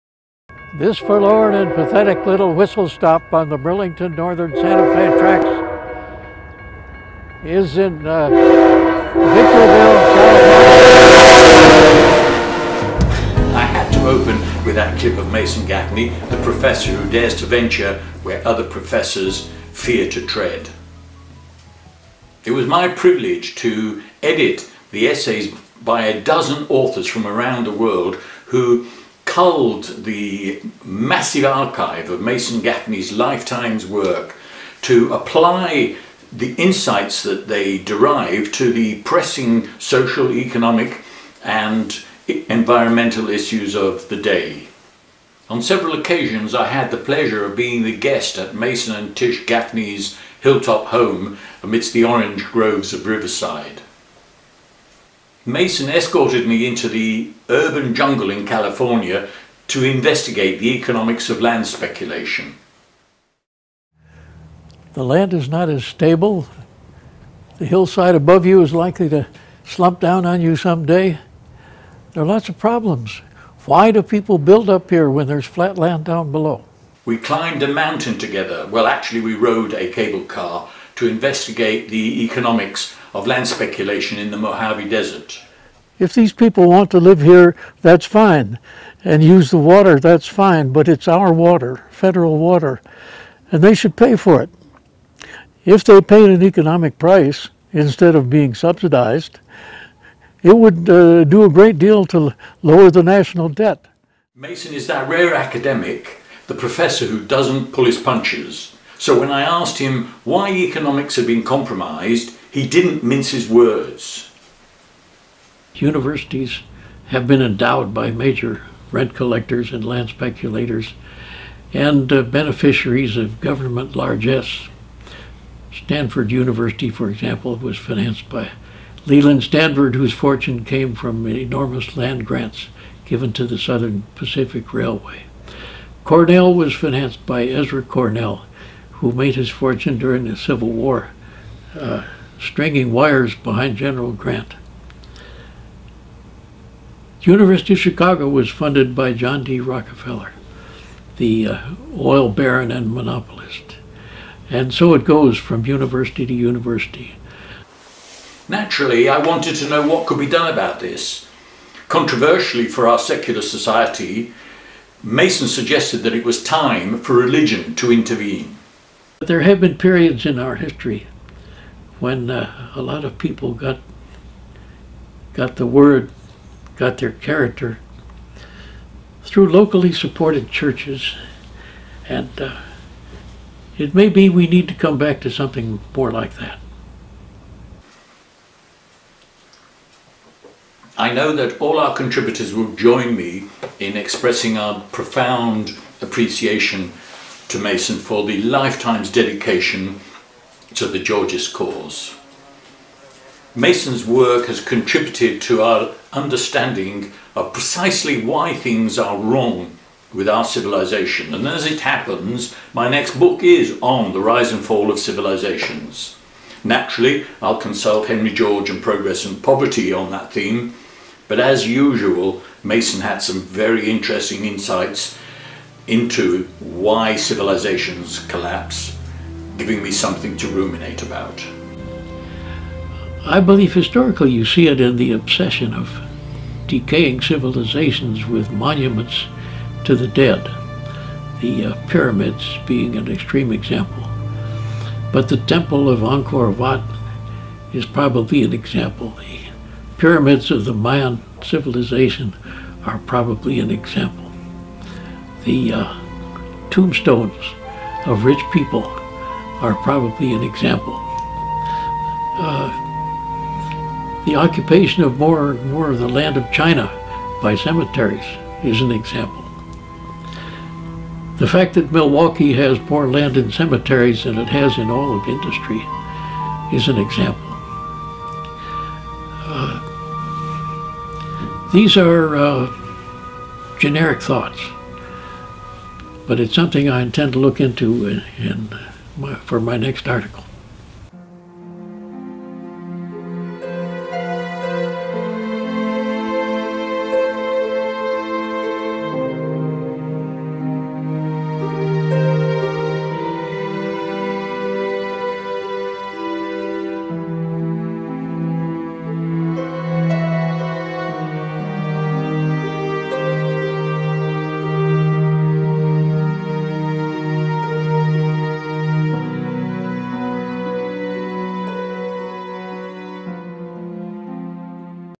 Economic Paralysis and its Antidote
The global economy is paralysed. The trap was laid by the land-led boom/bust speculation of the last property cycle. The escape route is traced by the authors of Rent Unmasked, a new book published to honour Mason Gaffney . Prof. Gaffney combined new theoretical insights with classical economics to develop strategies for redesigning the architecture of the market economy.

Rent Umasked is publish by Shepheard-Walwyn in association with the Robert Schalkenbach Foundation & can be purchased